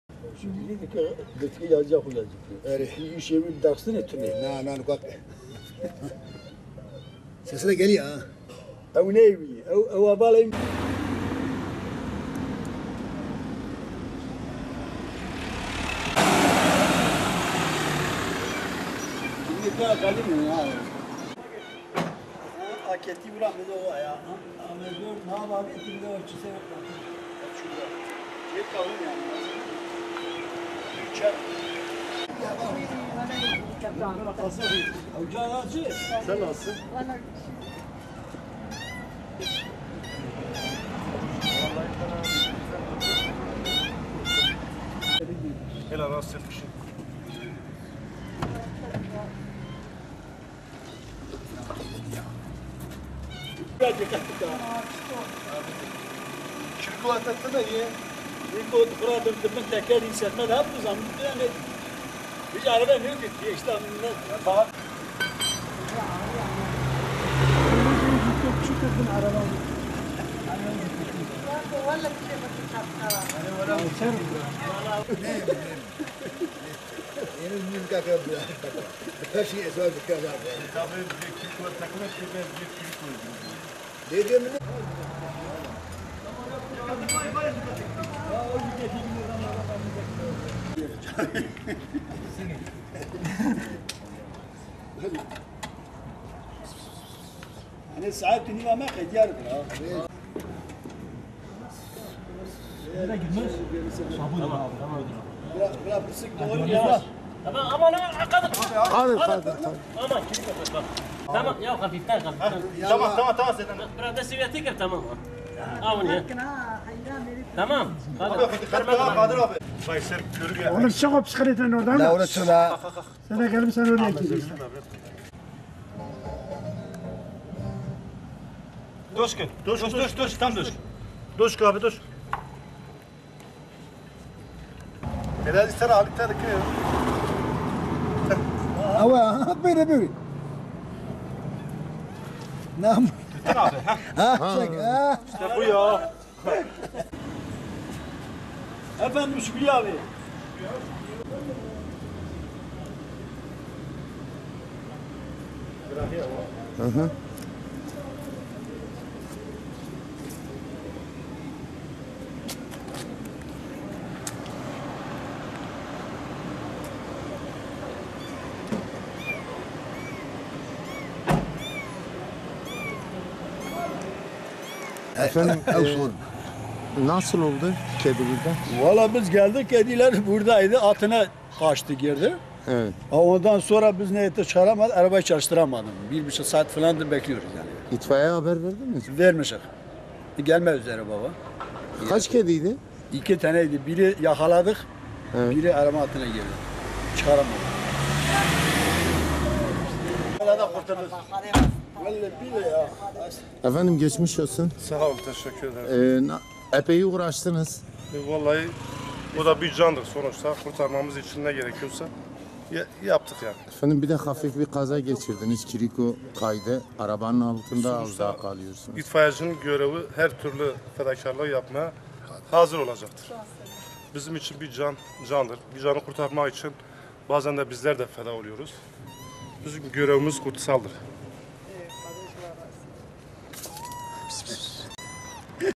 Siverek Son Haber - Otomobilin motoruna sıkışan yavru kedi kurtarıldı
Otomobilin motoruna sıkışan yavru kedi kurtarıldı